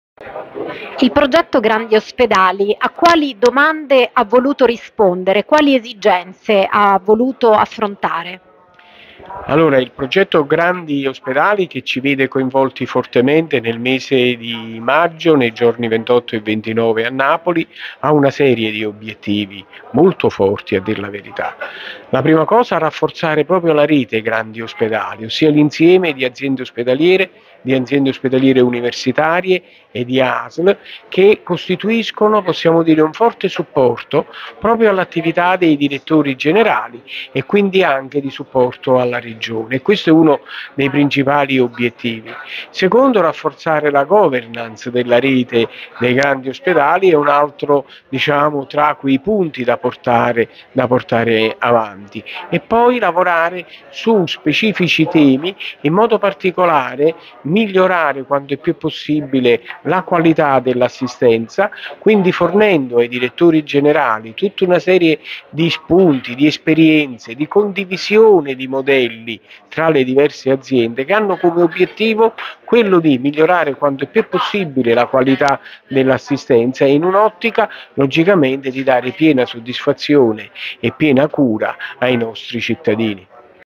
Roma - SANITA'. GRANDI OSPEDALI, LONGO: RAFFORZARE RETE PER I CITTADINI (20.02.24)
- Roma - SANITA'. GRANDI OSPEDALI, LONGO: RAFFORZARE RETE PER I CITTADINI
"Rete", "rafforzare la rete della governance" "migliorare la qualità dell'assistenza ai cittadini". Sono i tre concetti chiave, anima del progetto Grandi Ospedali, che alla Dire ha presentato Giuseppe Longo, Direttore Generale dell' AOU Federico II, Napoli, a margine della conferenza stampa di oggi al ministero della Salute (20.02.24)